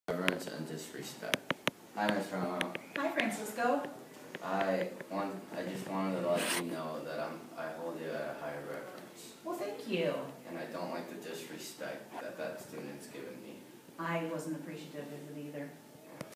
ELL 6th grade Vocabulary Antonyms- Reverence & Disrespect
Francisco talks to teacher Mrs. Frommelt about the words reverence and disrespect.